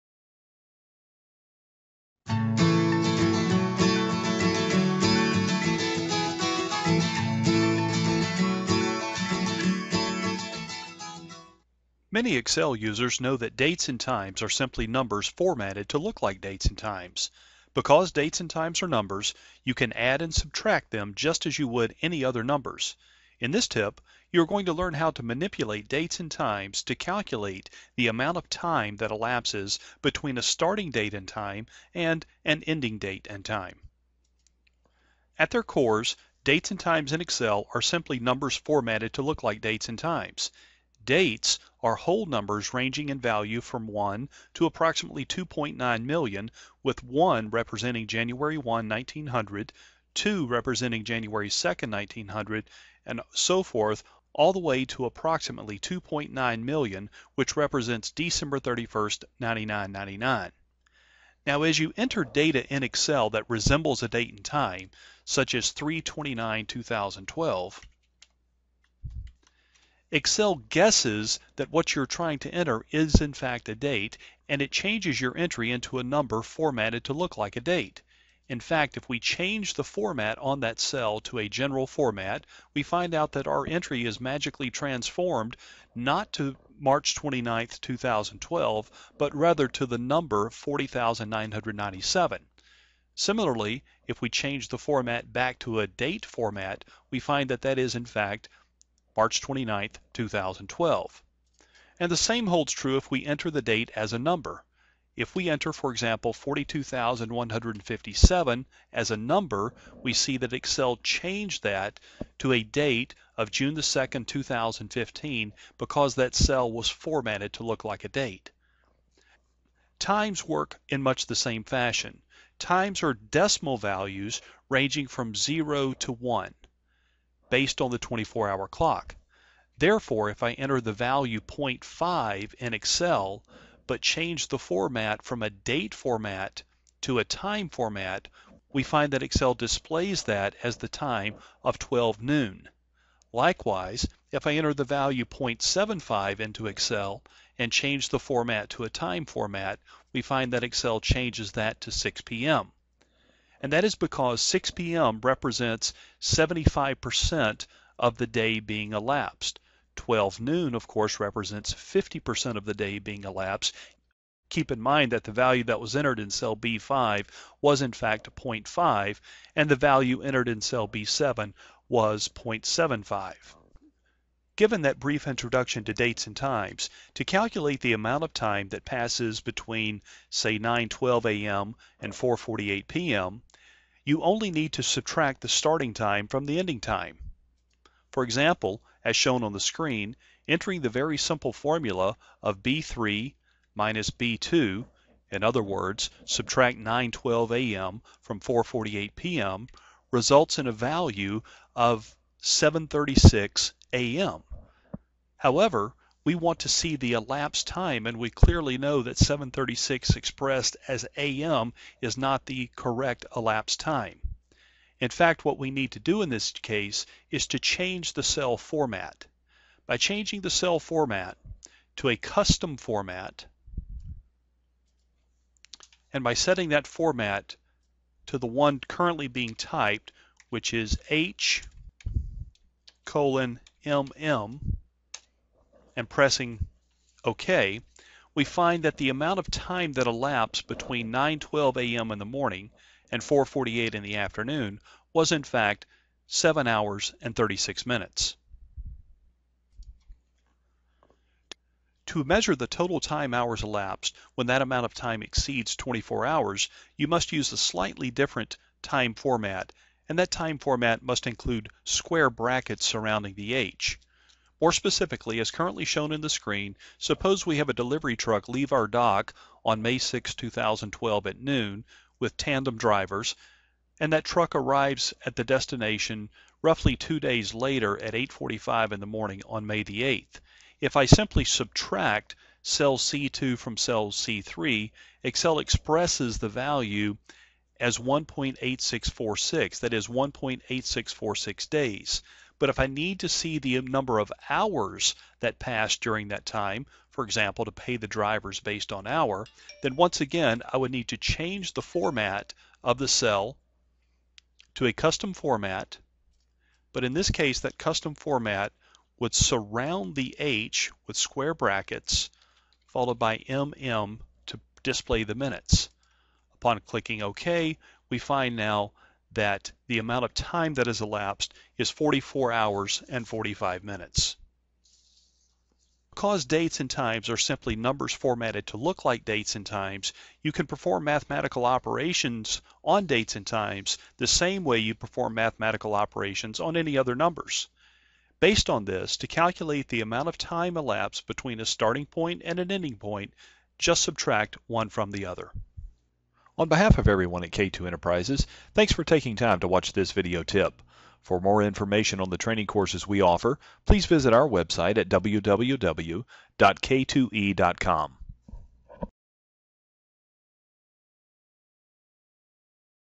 Calculating The Total Amount Of Time Elapsed In Excel
Many Excel users know that dates and times are simply numbers formatted to look like date and times. Because dates and times are numbers, you can add and subtract them just as you would with any other numbers. In this tip, you will learn how to manipulate dates and times to calculate the amount of time that elapses between a starting date and time and an ending date and time.